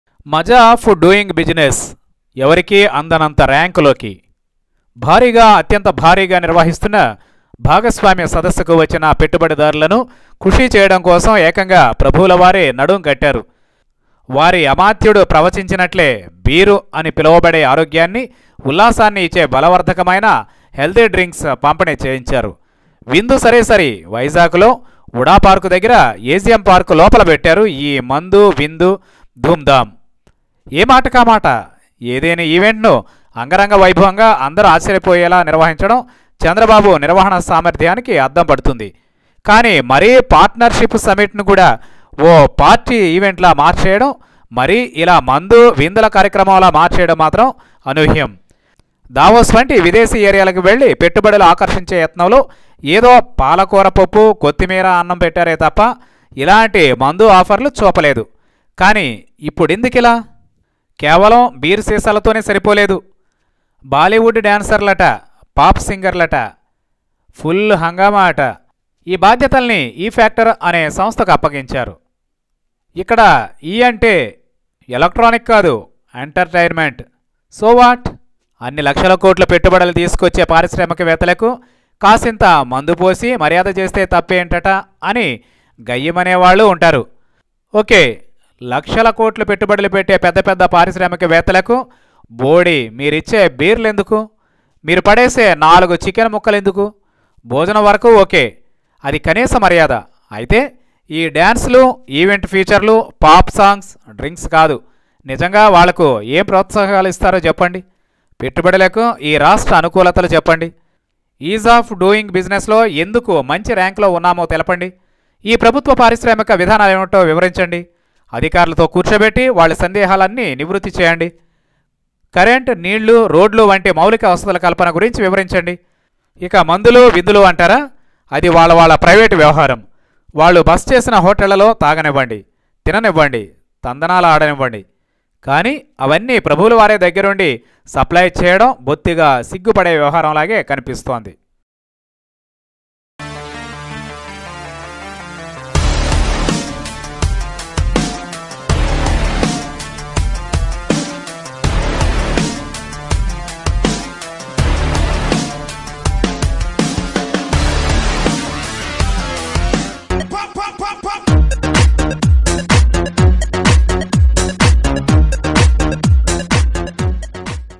చంద్రబాబు మందువిందుపై నెటిజన్ల సెటైర్లు| Criticisms on CM Chandrababu's Liquor Party in Vizag Summit
Criticisms on CM Chandrababu's Liquor Party in Vizag Summit
AP Government Liquor Arrangements to Vizag Partnership Summit Delegates Become Controversy. Netizens has made Setairs on the even.